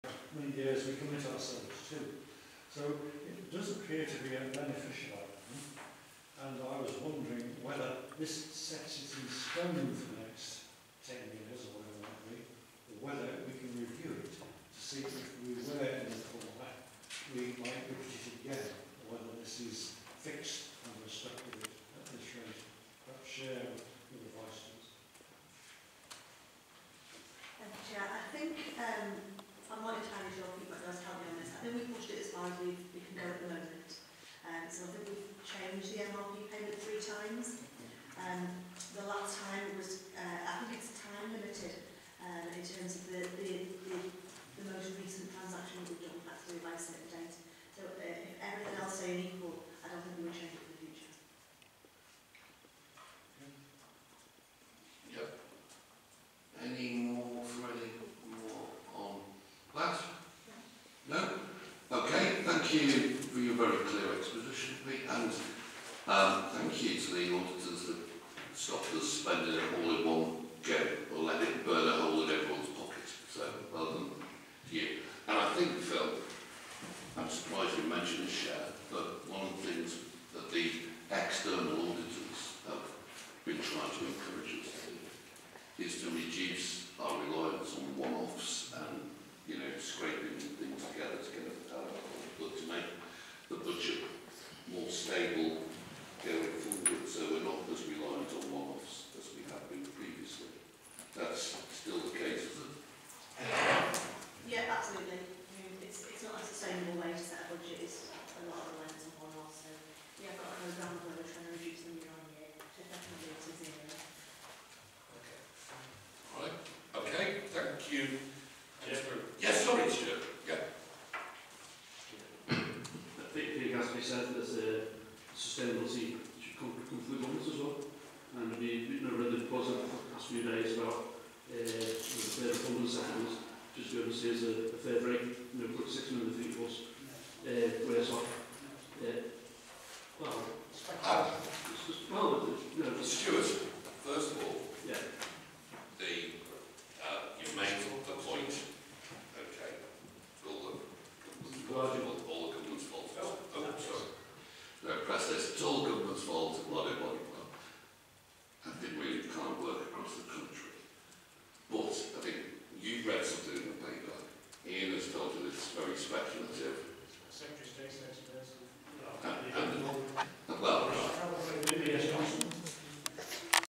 Audit and Risk Management Committee (Wirral Council) 27th January 2019 Part 3 of 5
Present:
Cllr Jeff Green (Chair)
Cllr Tom Anderson
Cllr Phil Gilchrist
Cllr Ian Lewis
Cllr Jean Robinson
Cllr Gillian Wood
Cllr Kathy Hodson
Cllr Kate Cannon
Cllr Adrian Jones
Cllr Stuart Whittingham

The agenda and reports for this meeting are on Wirral Council's

Continues from Audit and Risk Management Committee (Wirral Council) 27th January 2019 Part 2 of 5

AGENDA

6. MINIMUM REVENUE PROVISION RE-PROFILING (Pages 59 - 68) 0:01

Continues after a battery change at Audit and Risk Management Committee (Wirral Council) 27th January 2019 Part 4 of 5